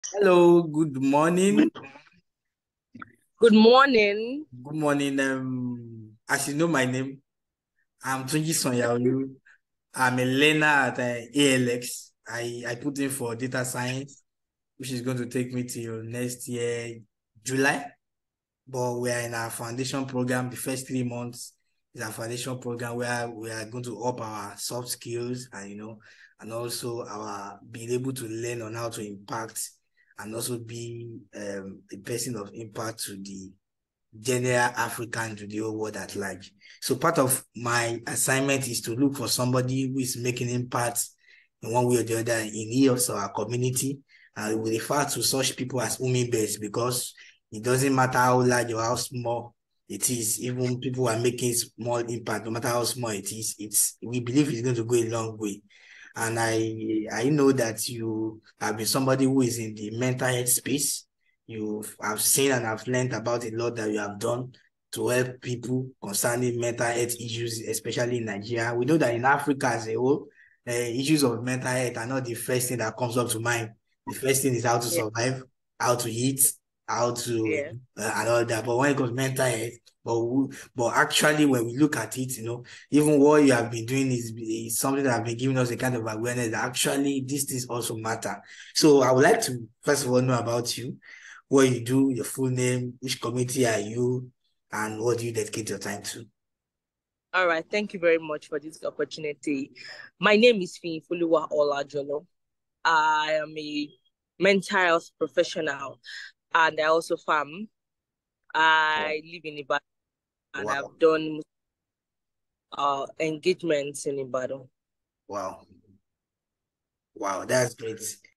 My Hummingbird Interview With Mental Health Professional
This 2-minute video is the introductory part of a 16-minute interview section held via Zoom video conference on Friday, June 16, 2023. The discussion is on the impacts made by Fiyinfoluwa Olajolo on mental health awareness within her community in Ibadan and globally via virtual training and seminars. She is a hummingbird and I am nominating her for the ALX hummingbird challenge.